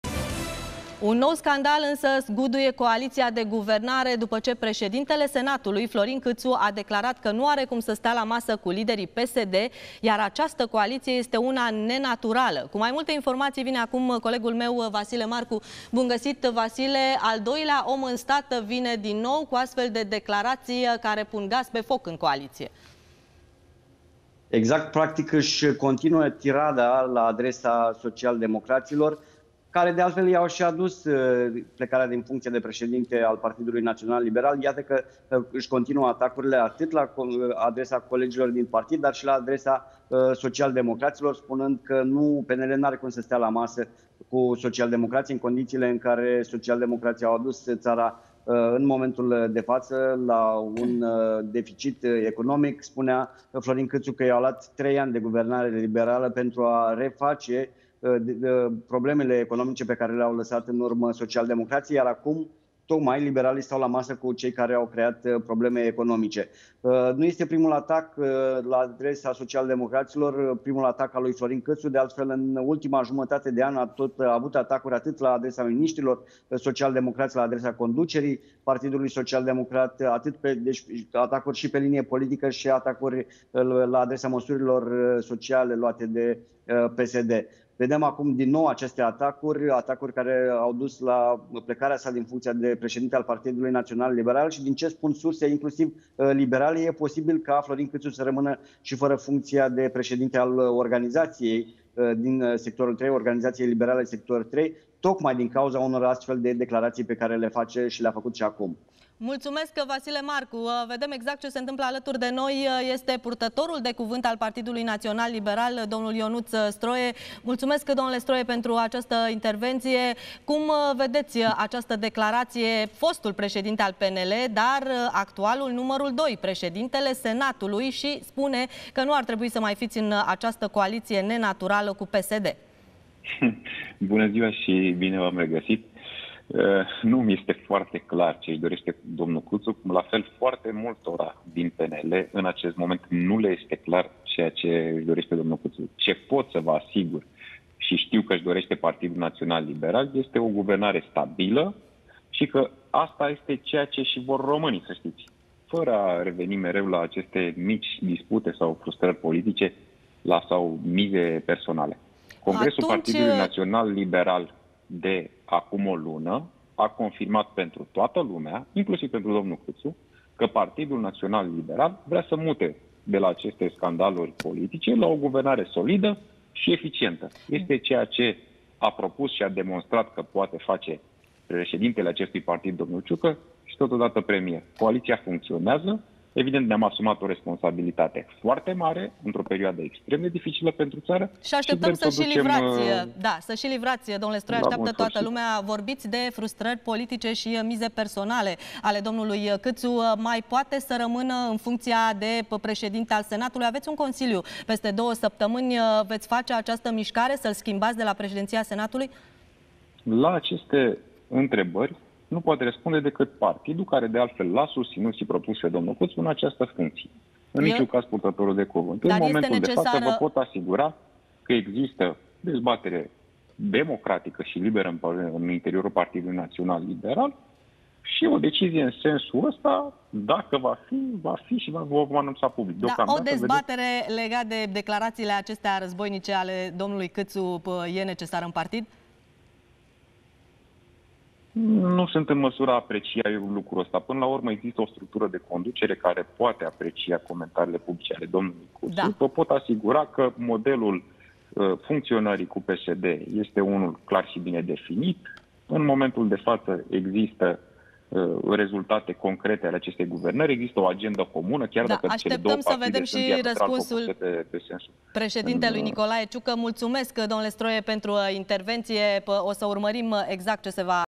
Un nou scandal zguduie coaliția de guvernare
Un nou scandal zguduie coaliția de guvernare după ce președintele Senatului, Florin Cîțu, a declarat că nu are cum să stea la masă cu liderii PSD iar această coaliție este una nenaturală.